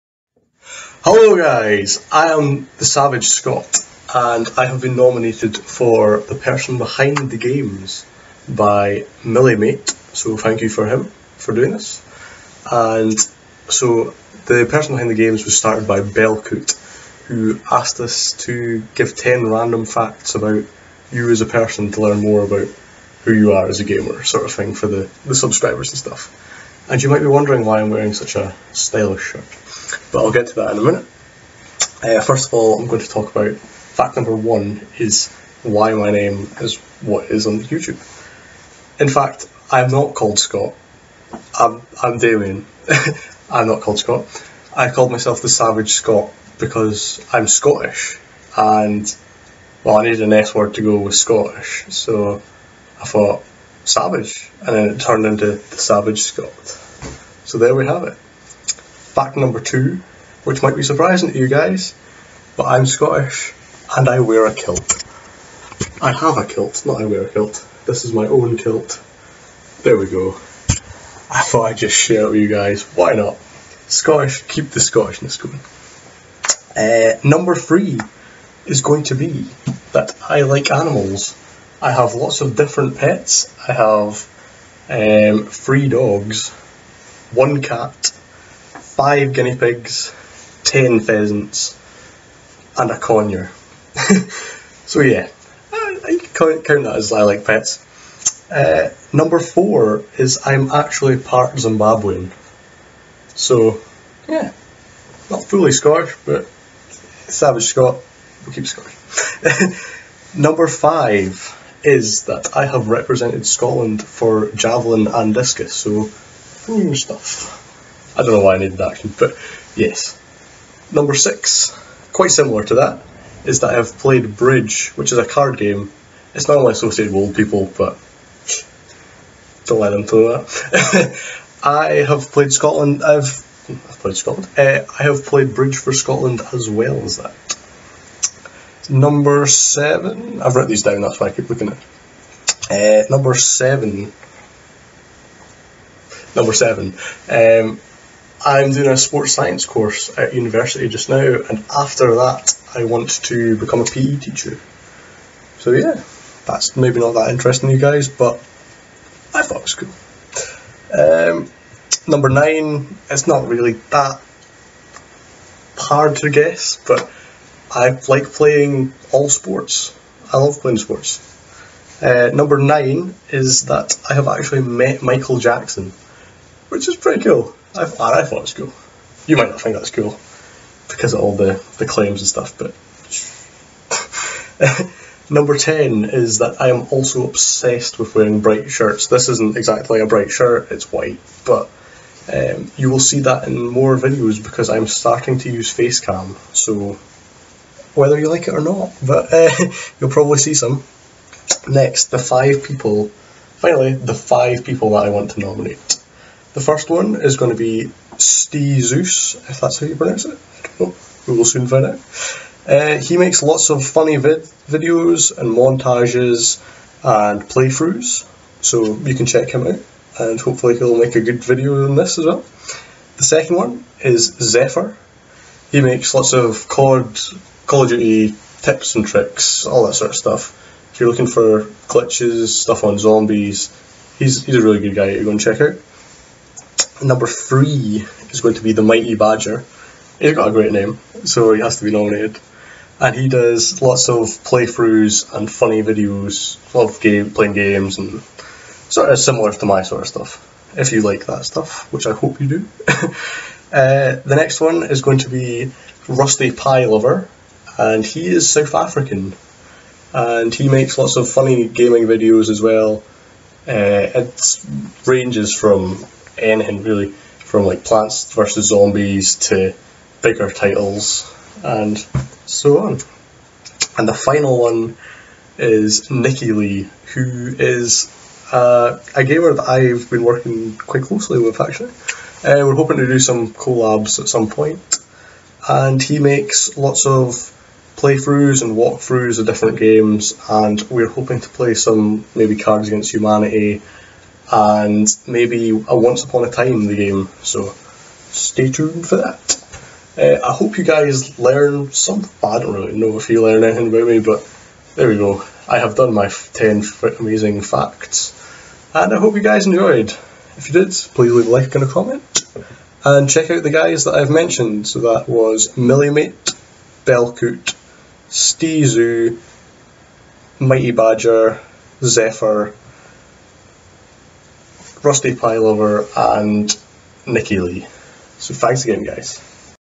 ThePersonBehindTheGames | Behind the Kilt
This is my first ever vlog because I was nominated to say 10 facts about myself to you guys so you can learn more about me by Milimate

Also please check out the 5 youtubers I have nominated which are